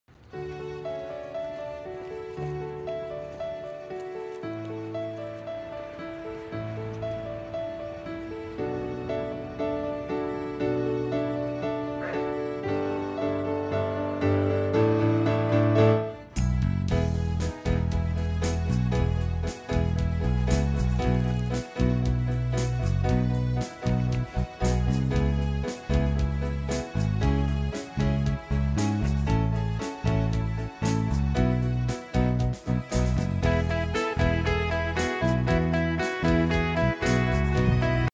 Corgi Love Story
Cocoa the Corgi walks by Luna's House every morning, but rarely gets to meet her due to work hour differences. Will today be any different? Let's see how their lovely story continues!